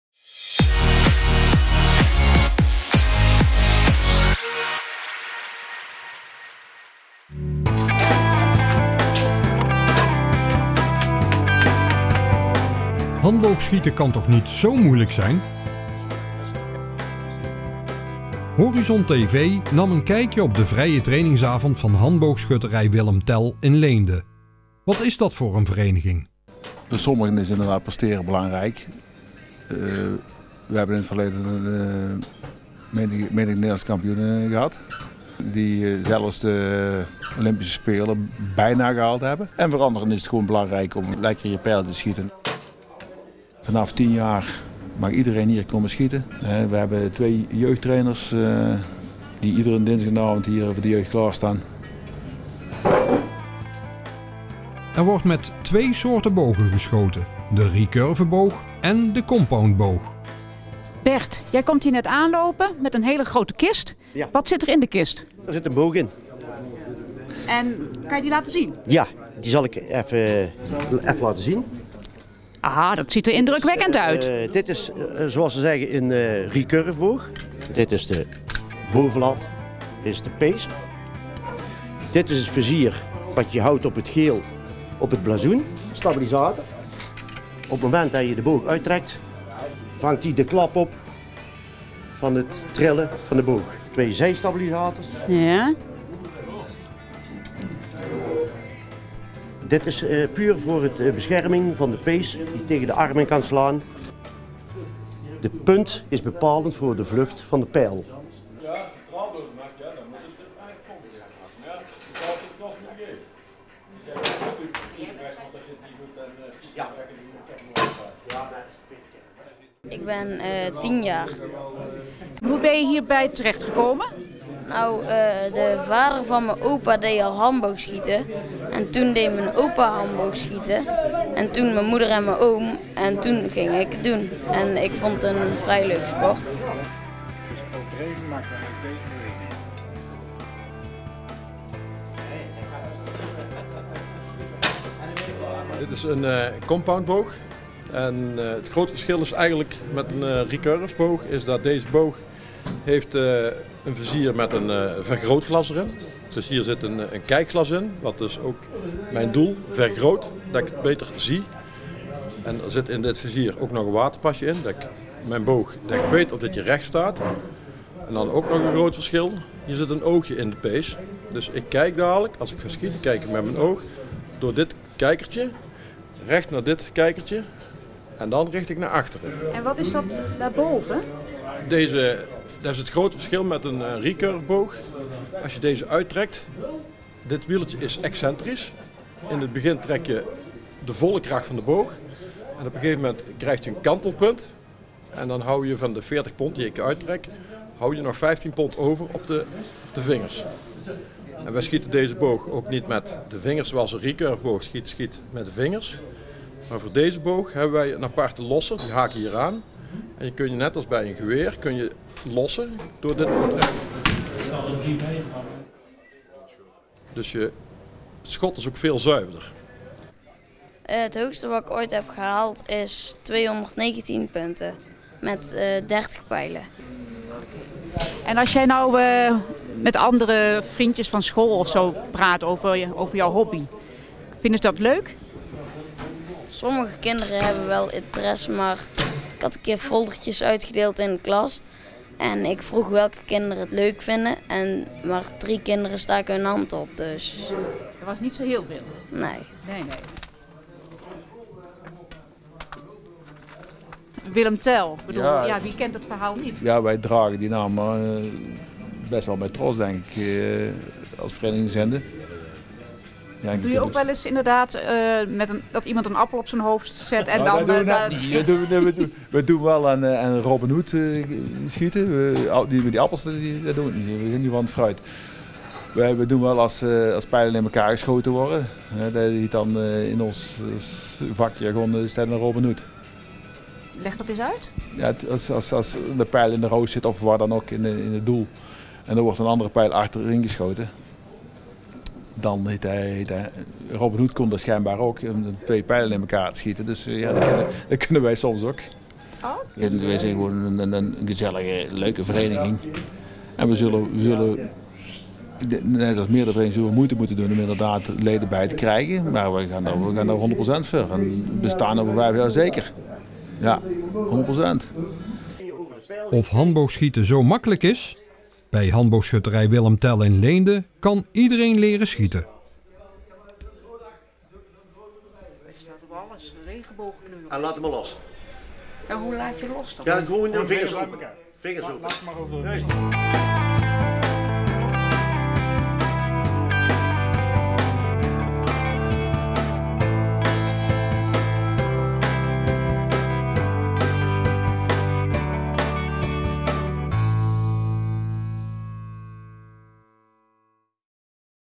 Dit is mijn club: Handboogschutterij Willem Tell in Leende
Meer weten over handboogschieten? Het adres van Handboogschutterij Willem Tell is Irislaan 1, 5595 EH Leende.